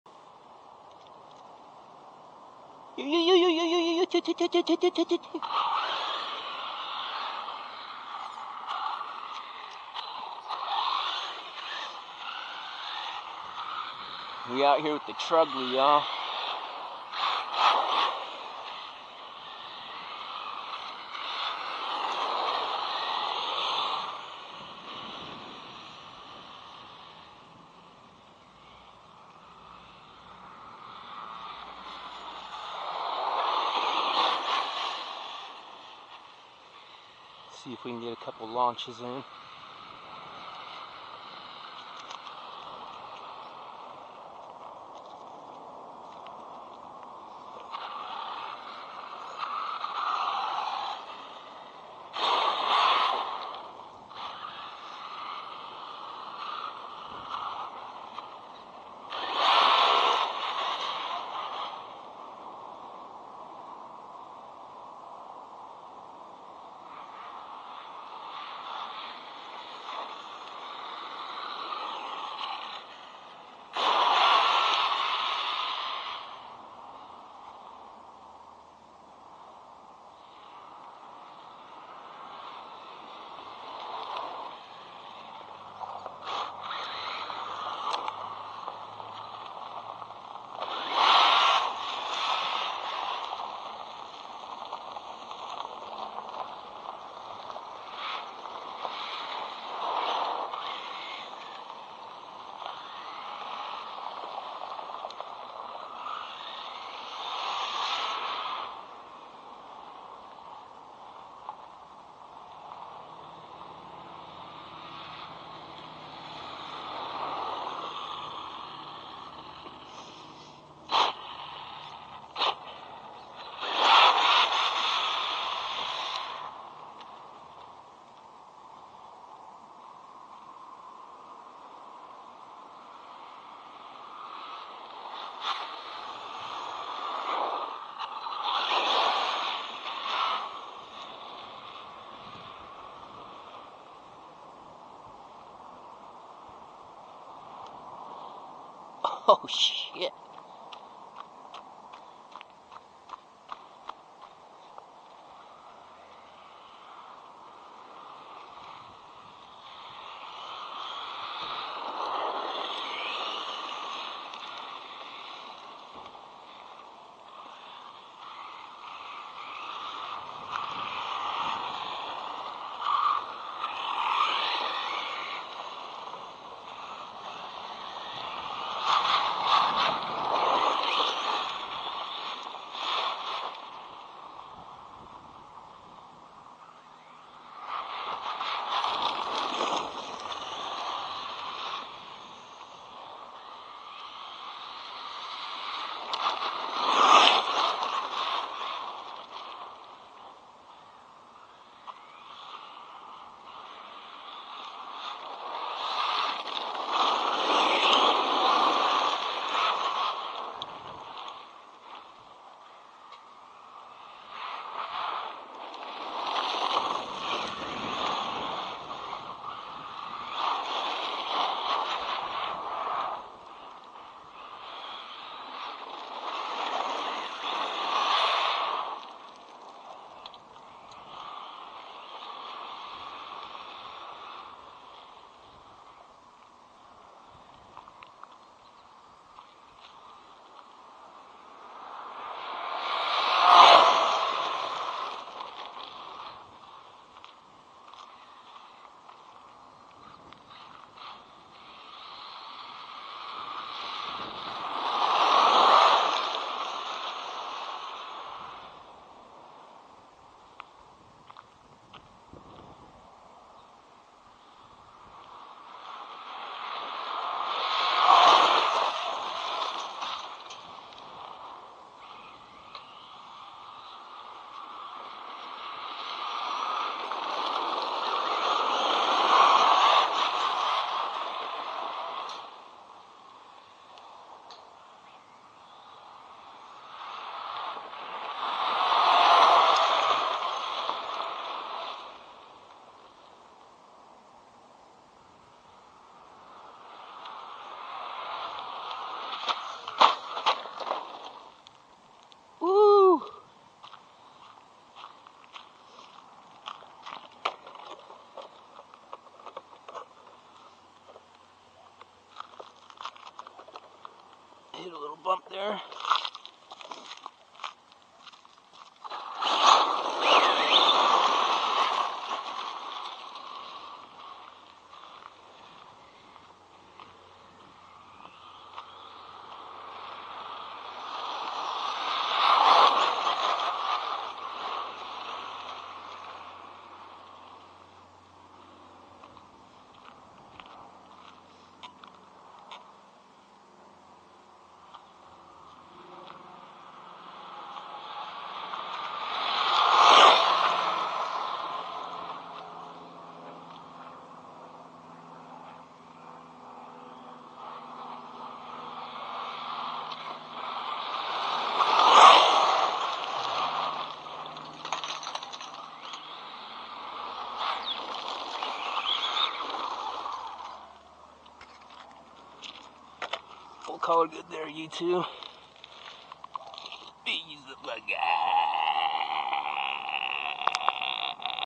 TeknoKiller Rippin The Truggly! Rock Shreddin 5s Monster!!! ft. Zd Racing Pirates 2 MT-8
Im back after a super psychotic move into a new place! Added a few more gray hairs 2 the collection but hey, I'm still here alive & bashing! Did a little trimming 2 the raptor F-150 body & voila!... tekno killer! lol This car iz simply amazing, it's like the first time driving it every time i take her out! Embrace the negativity & stay stoney world!!!